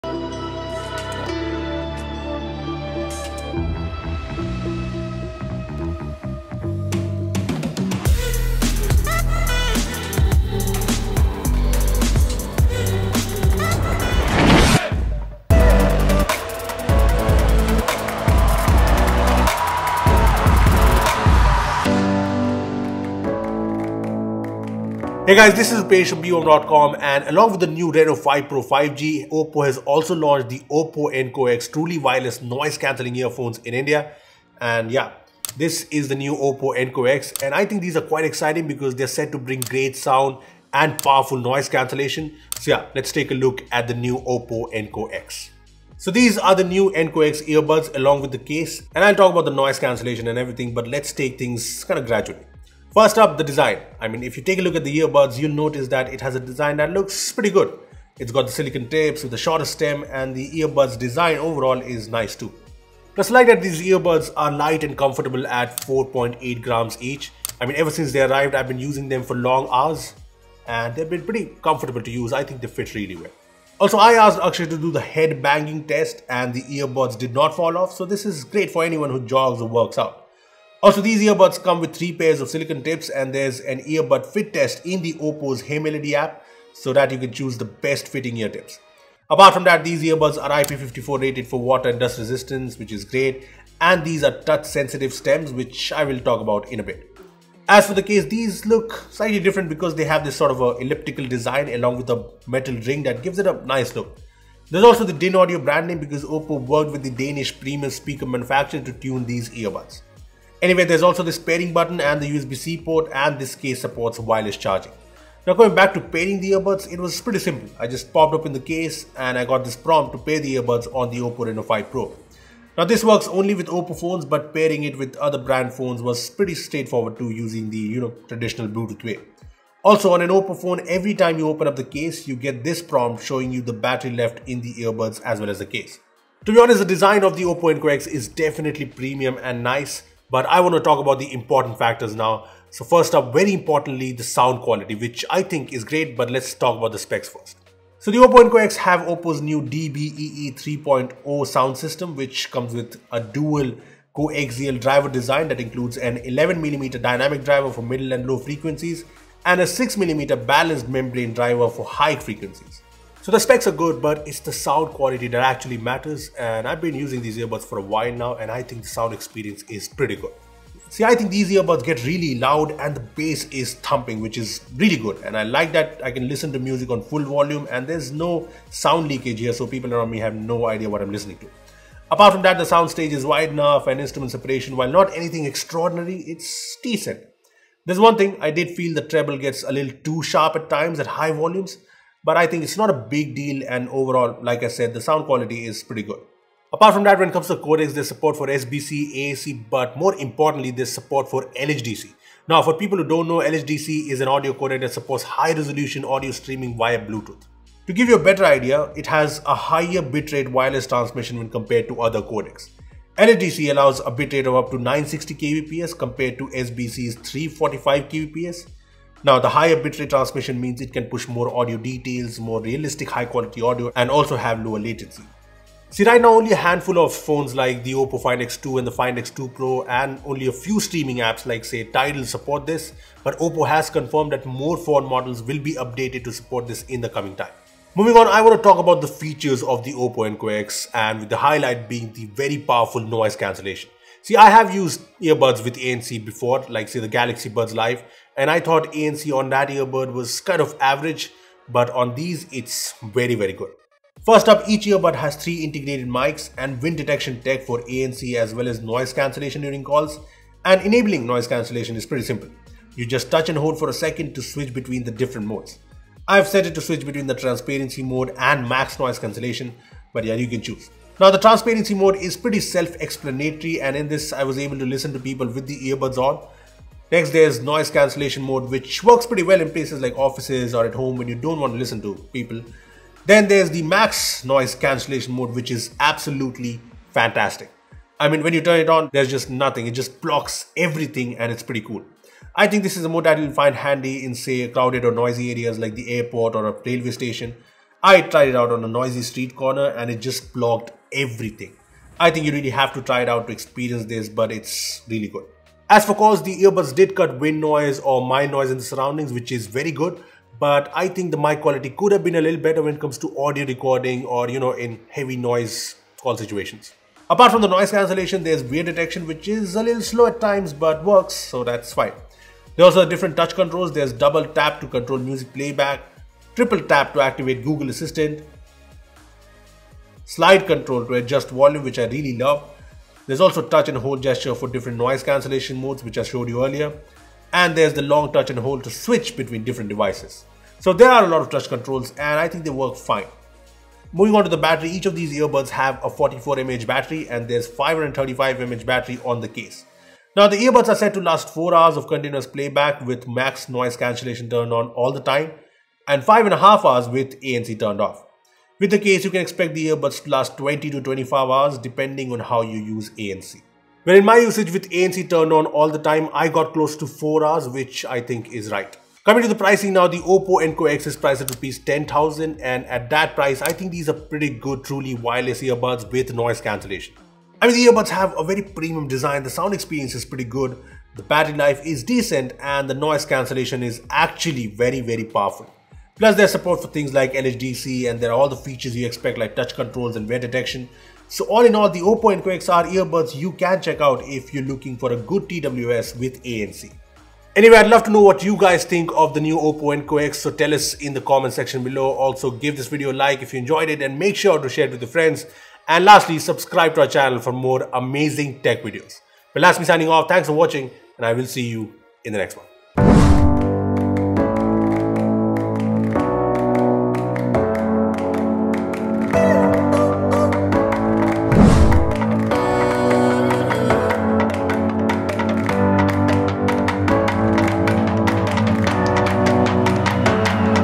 OPPO Enco X: The Ultimate ANC Experience!
Along with the new OPPO Reno5 Pro 5G, the company also launched the OPPO Enco X truly wireless earphones with noise cancellation. In this video, we talk about the OPPO Enco X's sound quality, the noise cancellation experience, features and more.